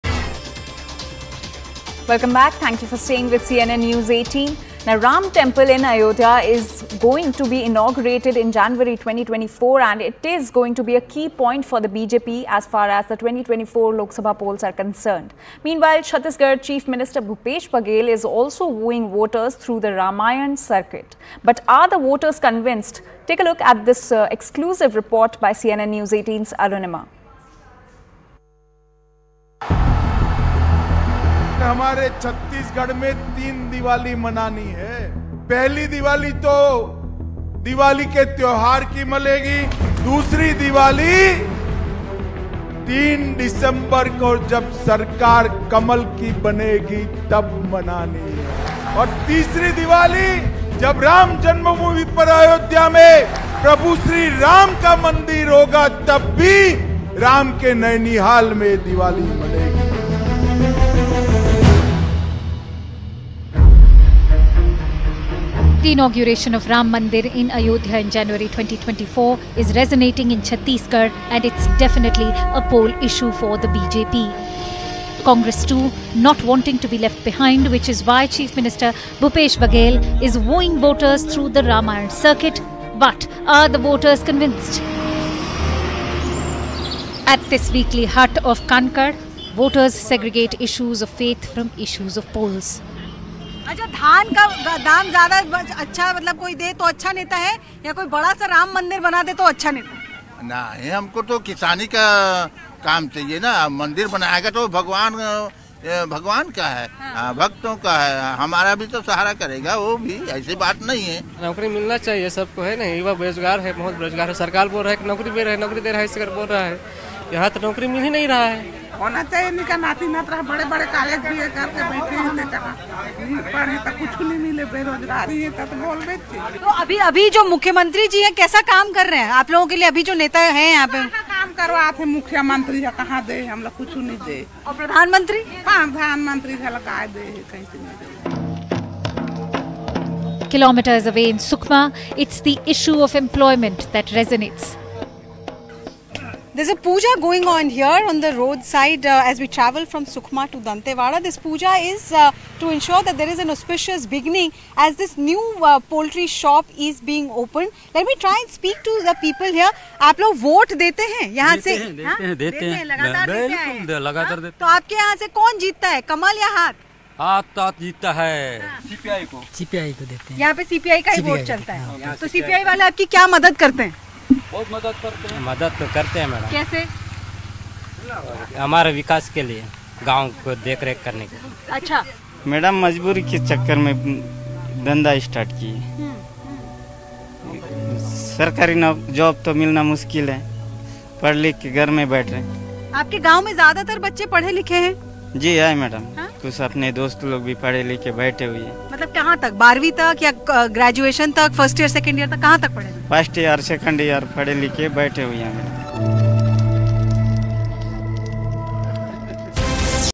Ram Mandir Inauguration 2023 | Ram Mandir In Ayodhya About To Be Inaugurated | News18
Ram Mandir in Ayodhya to be inaugurated in Jan 2024, is a poll issue for BJP.
CM Bhupesh Baghel is also wooing voters through the Ramayan circuit, but are the voters convinced?

n18oc_india